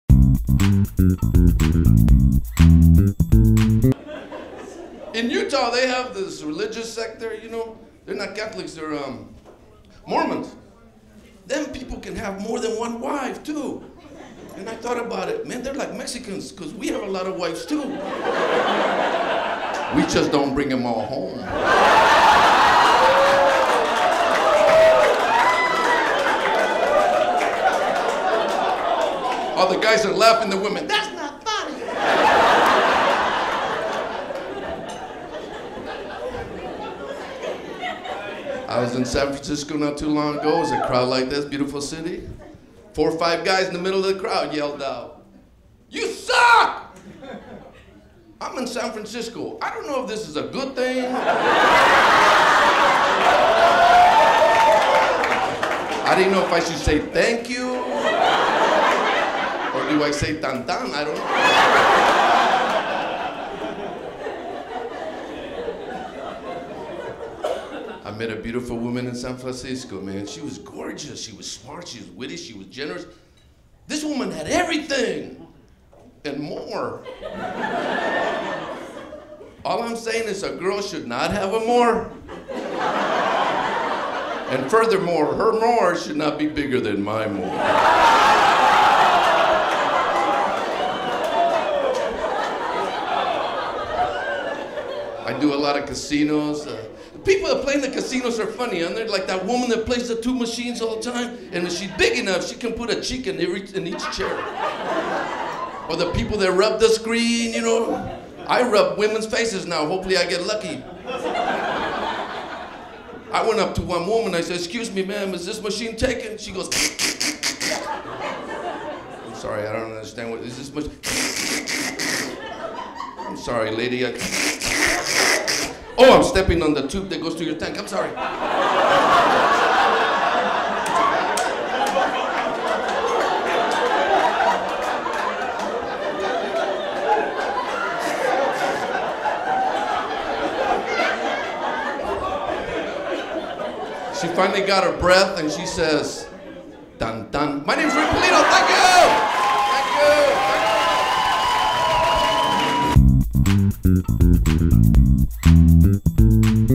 Mormons (Funny Videos)
Rick Pulido talks about the facts of Mormon life, and old ladies at slot machines.